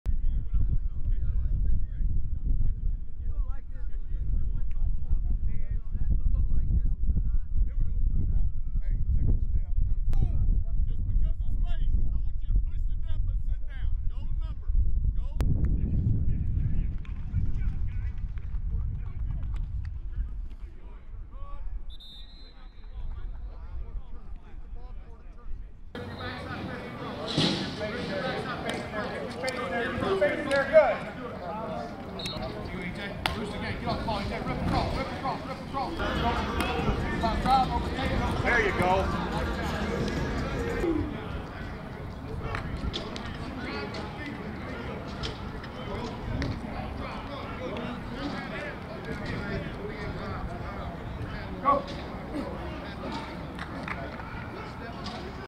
WATCH: Auburn Football hits the practice field for the first of the Hugh Freeze era | First of '23
Auburn Tigers videos from Auburn, Alabama from the staff of Auburn Live.